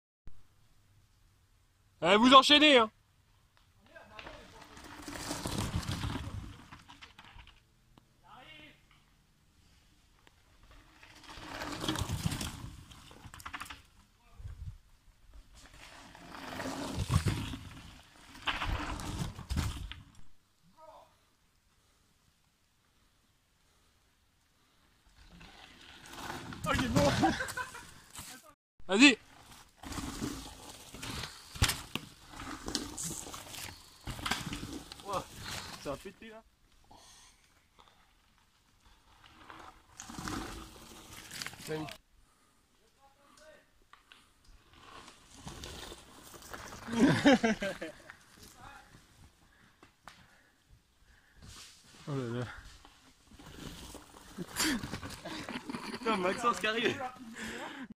VTT Saint Senoux 2019
Le club de Kayak de Guipry-Messac en Kayak à roues dans les trous de Saint senoux